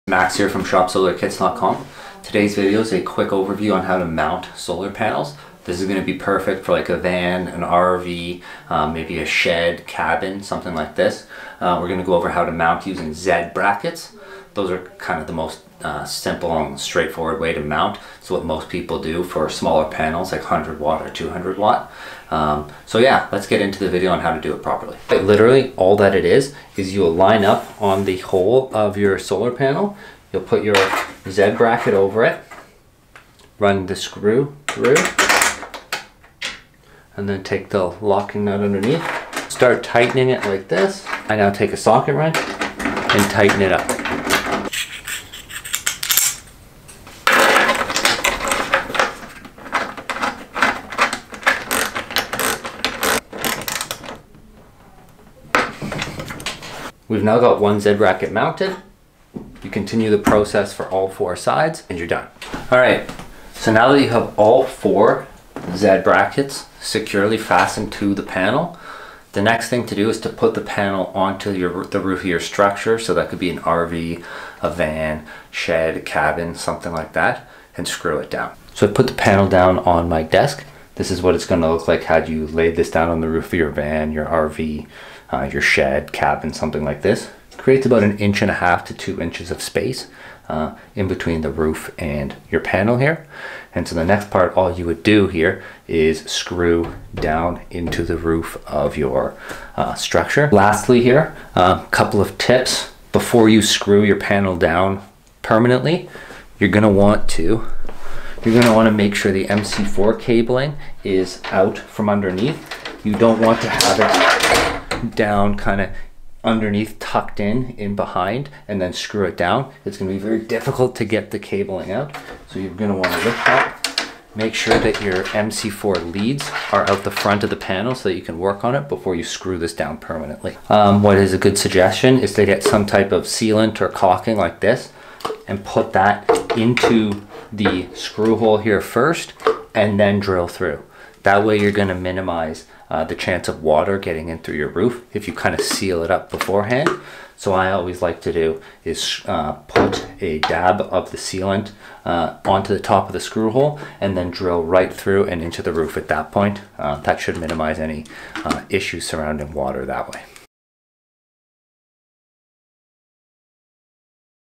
How To: Mounting Solar Panels
Get Instant Access To Our [DIY Solar Essentials] | 7 X Video Training Modules + 40 Page Solar Handbook | Delivered Straight To Your Inbox

In this video we show you how to mount a solar panel using standard Z-Brackets.  This is the most common way to mount panels to the roof of a Van, RV, Shed, Cabin etc.